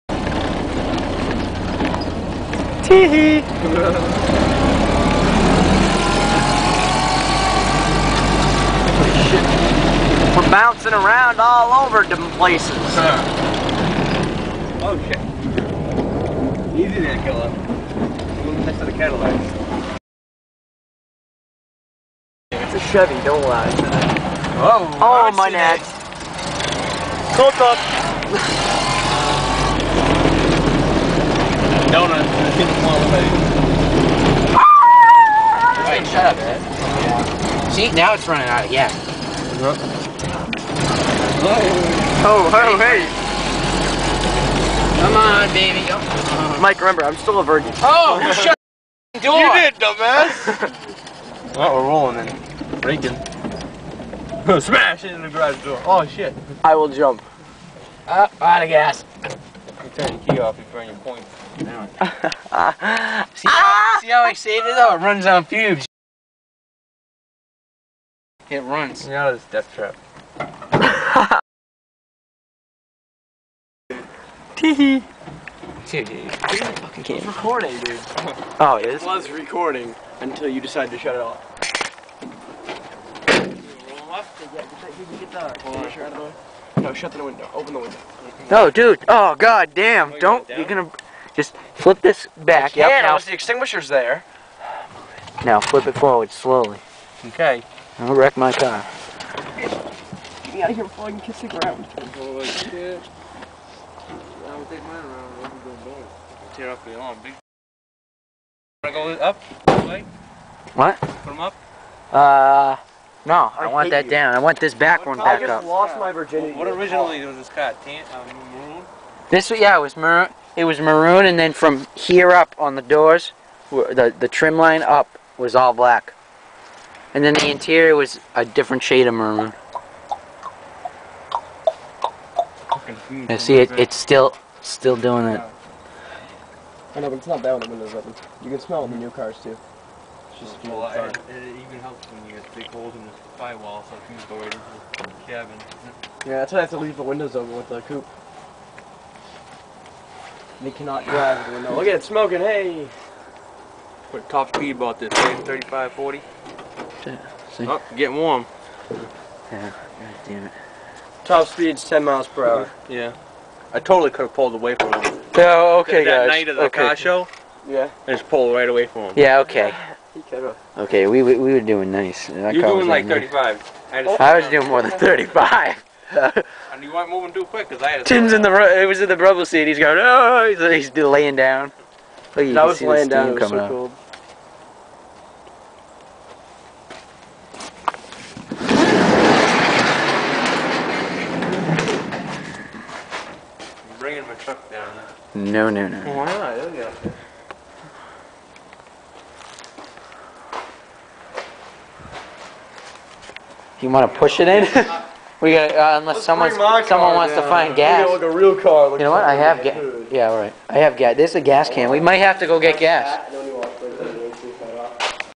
Driving around in the tudor w/ Tim and Justin and then..."oh shit dude we're out of gas"
from like october or september. just found  this one.